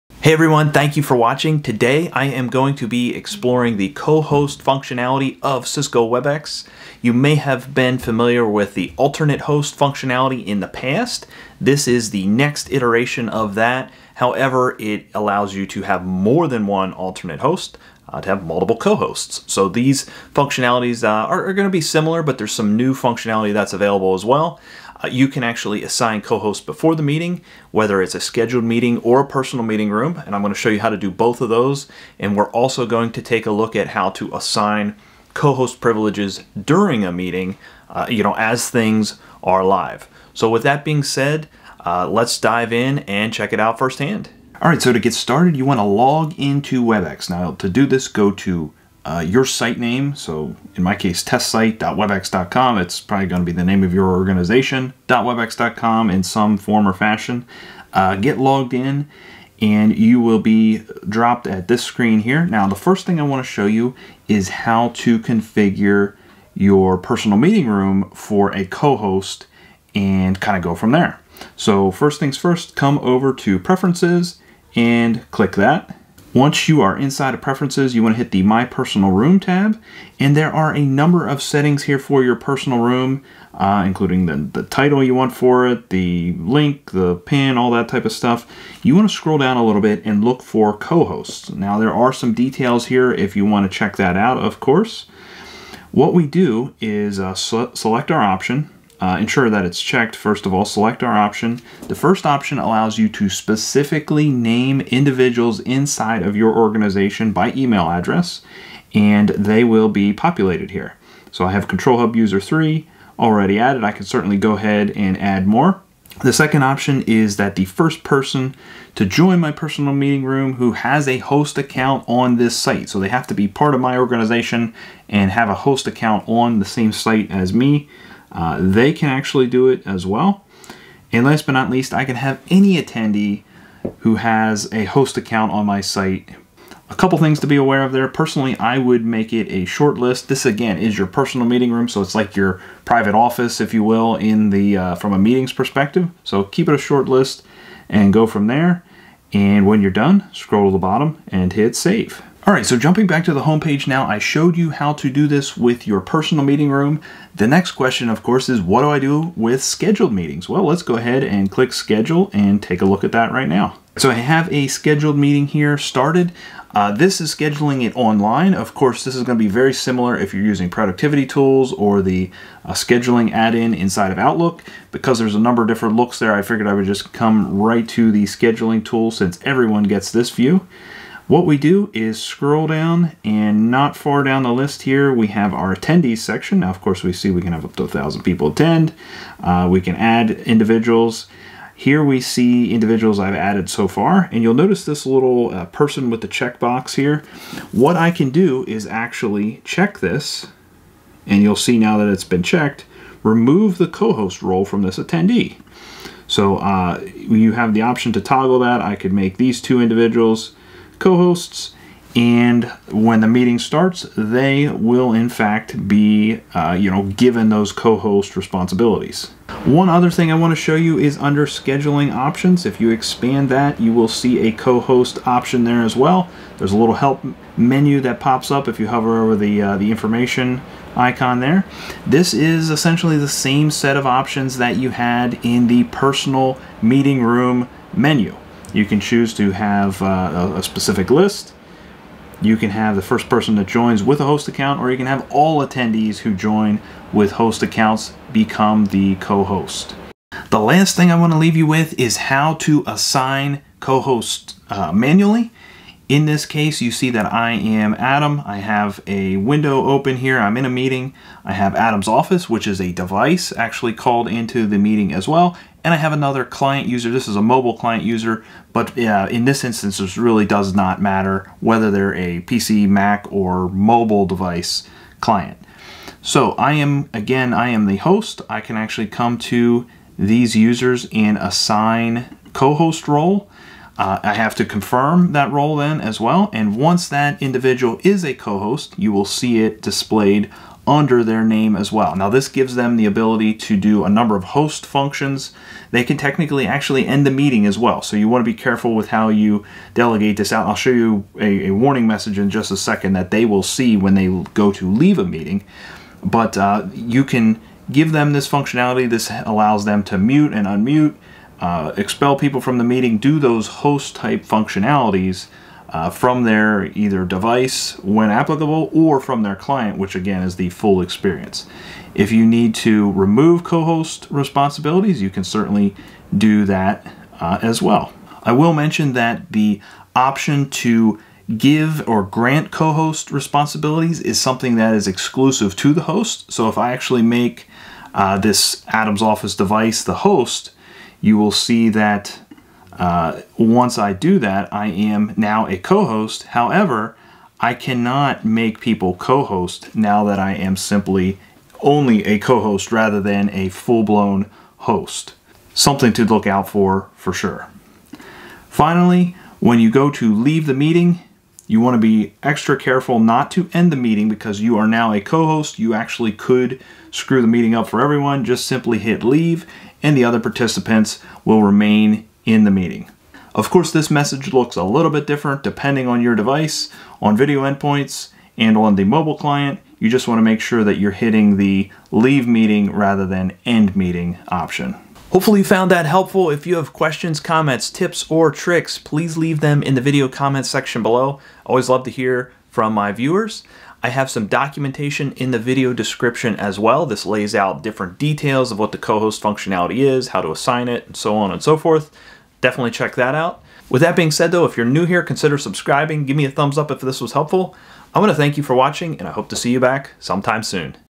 How To Use Webex Co Host in Personal Meeting Rooms, Scheduled Meetings and During Meetings
In this video I show you host to assign Co Host rights before the meeting and during the meeting.  Similar to how alternative host was assigned in the past you can assign cohosts before the meeting for both PMR and for scheduled meetings.  I then share how you can assign Co Host during a meeting as well.  Finally, there are a few things to keep in mind to prevent ending the meeting accidentally.